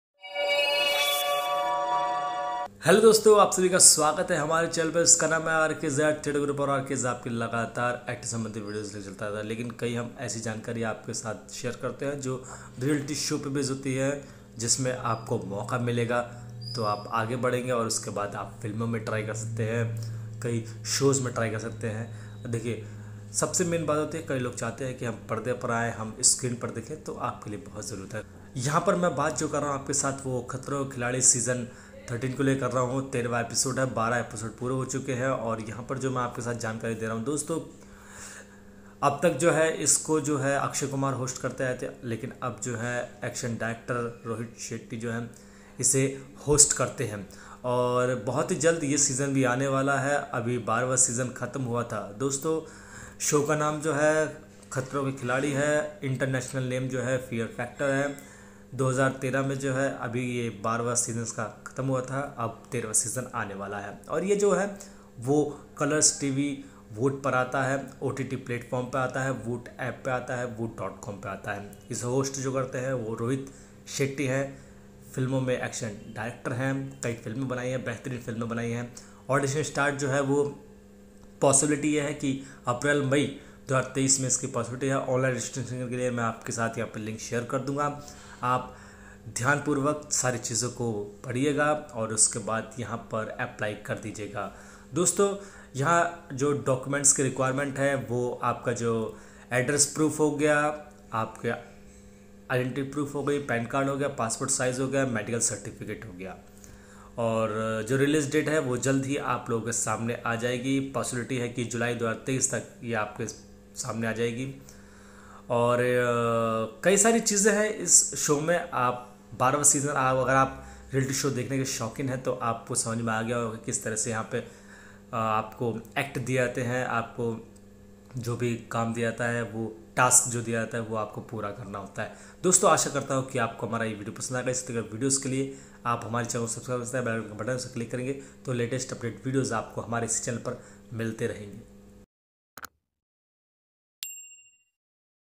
Actor's Apply For Khatro Ke Khiladi | Khatro ke khiladi Audition | Fear Factor | Rkz Theatre
Welcome Our Online Acting Youtube Channel

Hello Friends

you Can Apply For Khatro ke khiladi season 2023
Actor's Apply For India Fear Factor

About Video- Khatro Ke Khiladi 2023